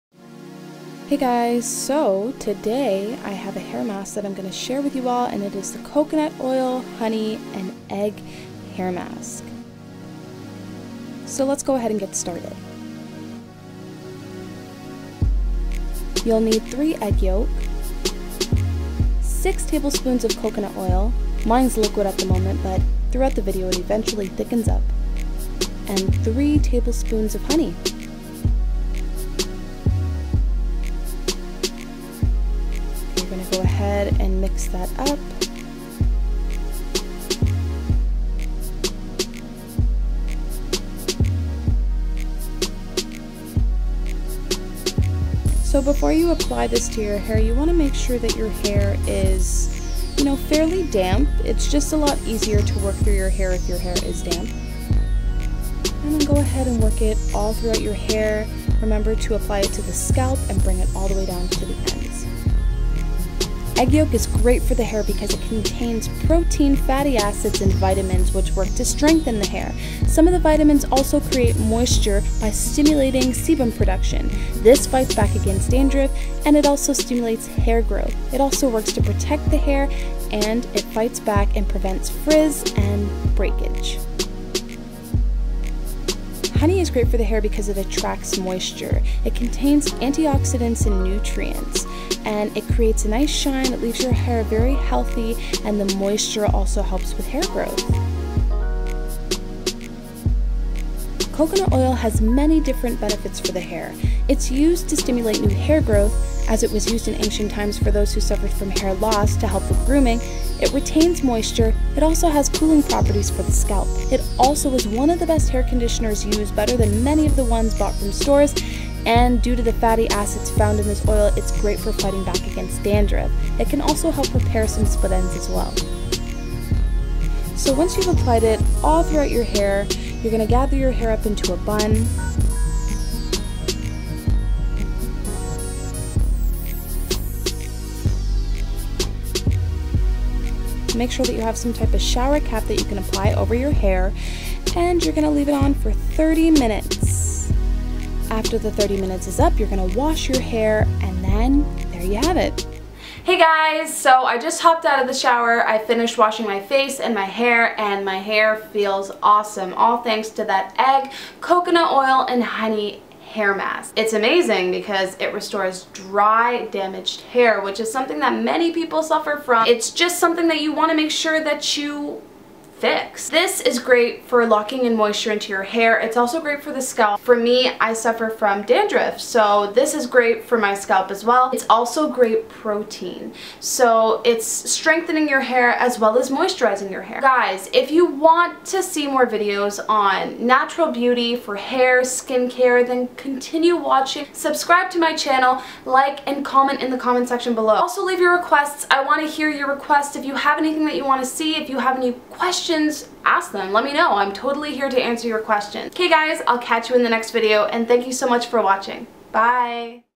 Natural Hair: Egg, Coconut Oil, Honey Hair Mask - For DRY DAMAGED HAIR
Hey lovelies!

  I hope you're all doing well. Today I'd like to share with you a hair treatment that I like to use for dry damaged hair. The Egg, Coconut Oil and Honey Hair Mask. This hair treatment has numerous different benefits for the hair.
1) Egg Yolk contains protein, fatty acids, and vitamins that aid with:
- Hair Strengthening
- Hair Moisture
- Hair Growth
- Dandruff
- Frizz
- Breakage
- Protection

2) Honey contains antioxidants and nutrients that aid with:
- Hair Moisture
- Shiny Hair
- Hair Growth

3) Coconut Oil contains vitamins and minerals that aid with:
- Hair Growth
- Hair Moisture
- Hair Conditioning
- Dandruff
- Split Ends

This combination works wonder for the hair if used consistently when a hair treatment is needed. By repairing dry damaged hair, you will soon see your hair start to take on a beautiful shine with some new growth as well. Learn how to grow your hair by starting with the root problem. Having a natural way to maintain healthy hair is a blessing. There are so many hidden gems right in your kitchen, hiding in your fridge and on your shelves that you may not even think of.